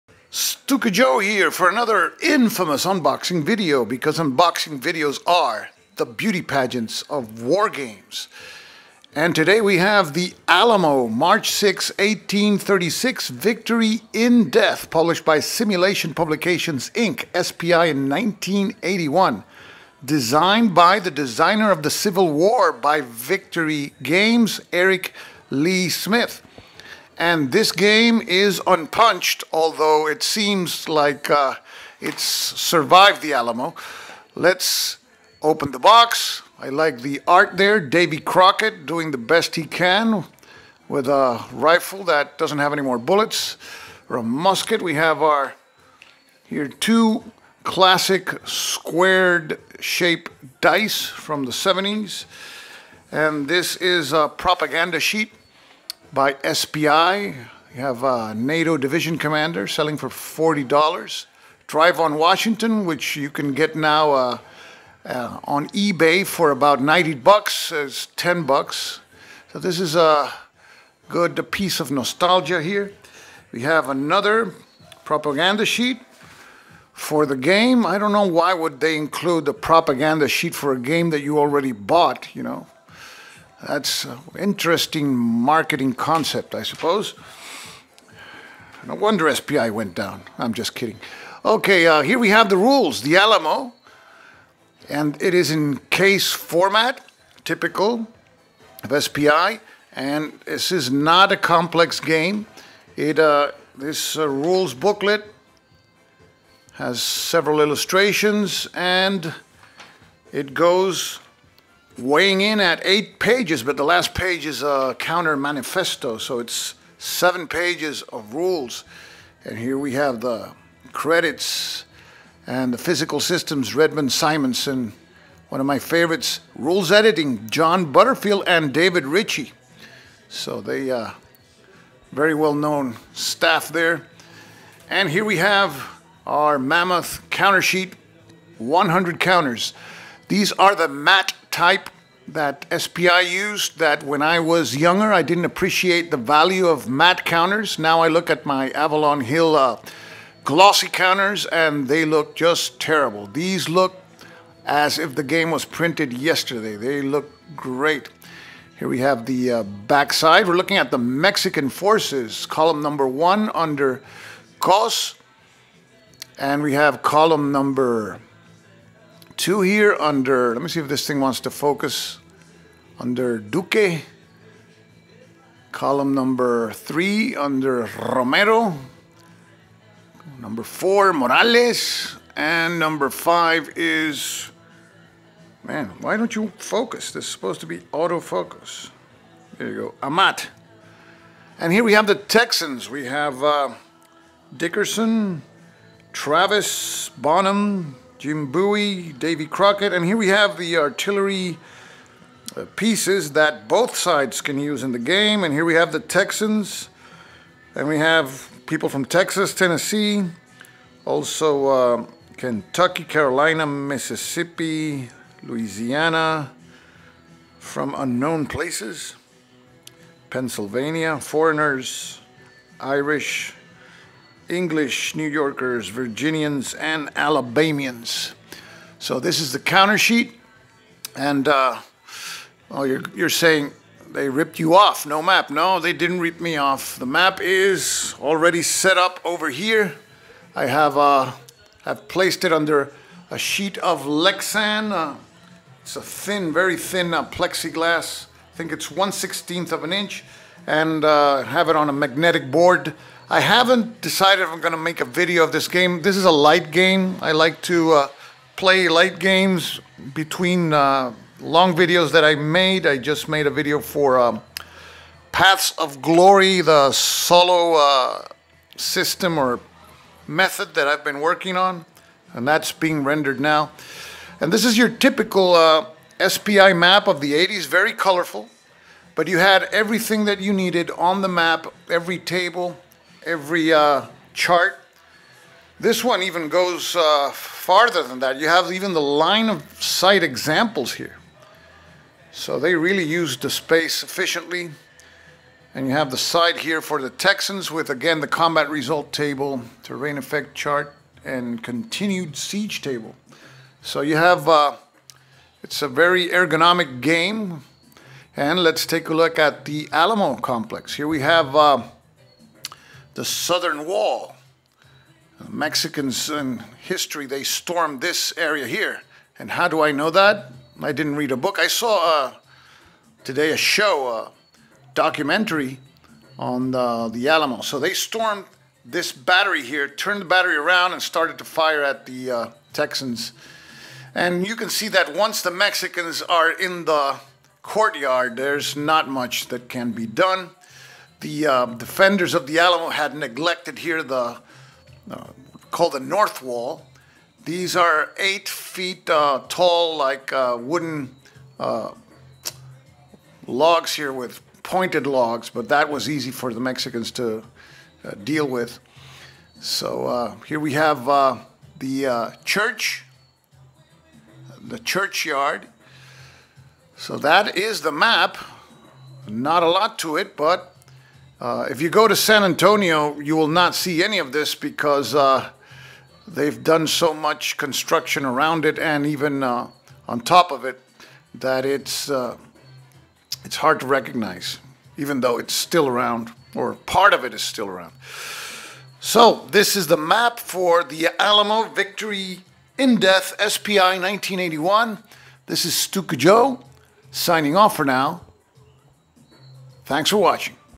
Informal Unboxing of The Alamo, Victory in Death
Informal unboxing video of The Alamo, Victory in Death, a game published by Simulations Publications, Inc., in 1981.